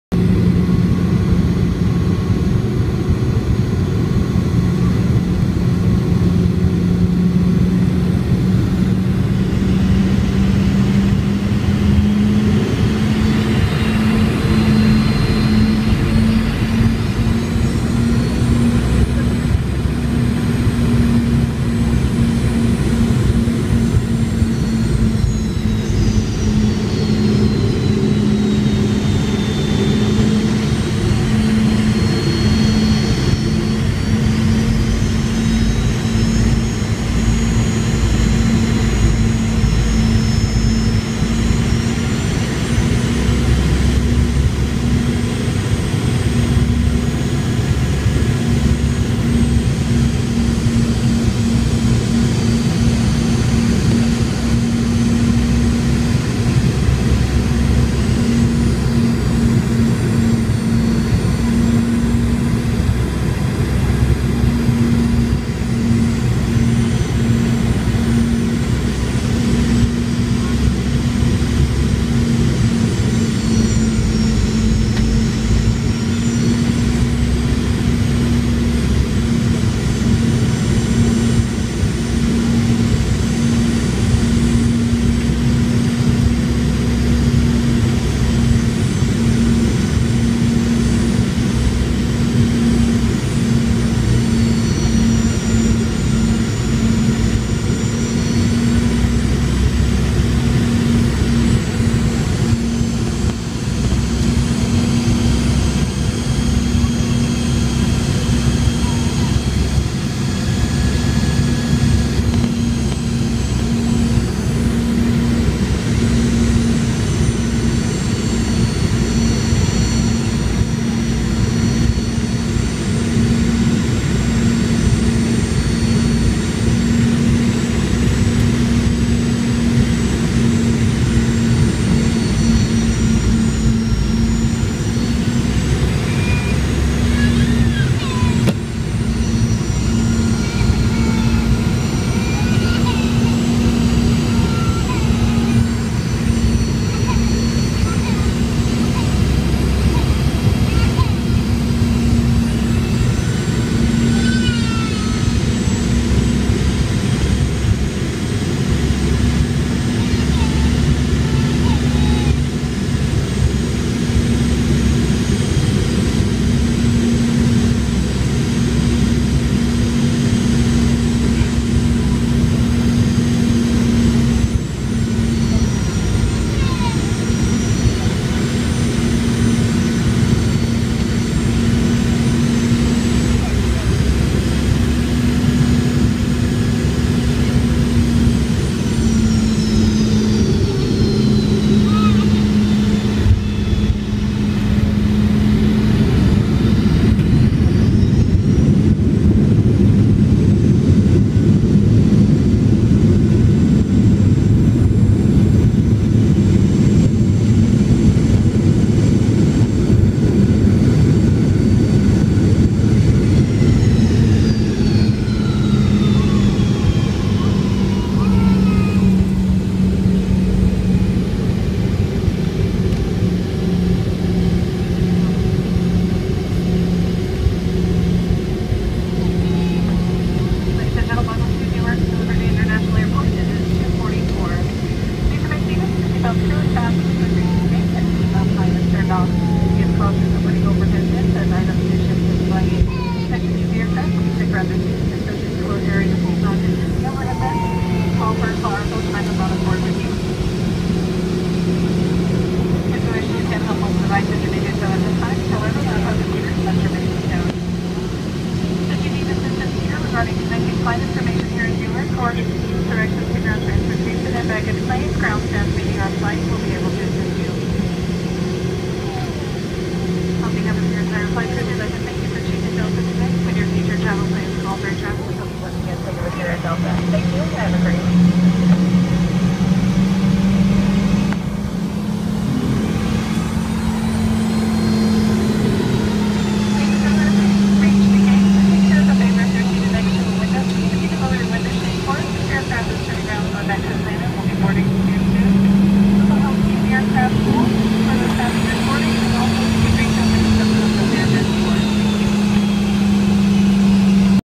Delta 717 landing
Arriving into EWR after an 2 hour ish flight from ATL. Enjoy everyone!